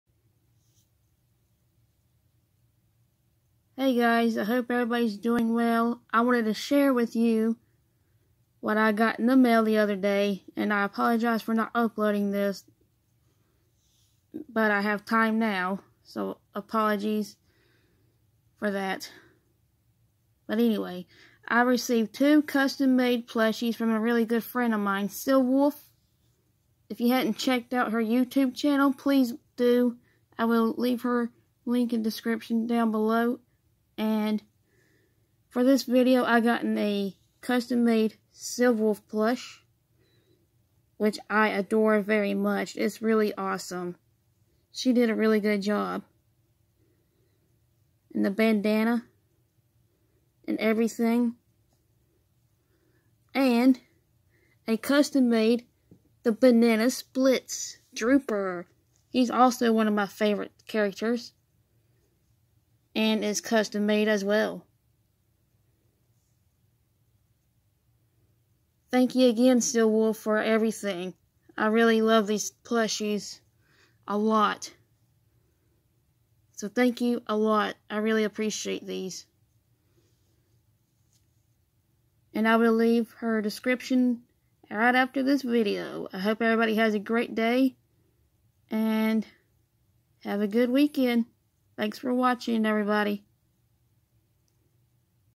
another awesome package from my friend silvolf 🤗
please check out my friends channel silvolf and store if you want a custom made plush she's a really good plushie maker 😁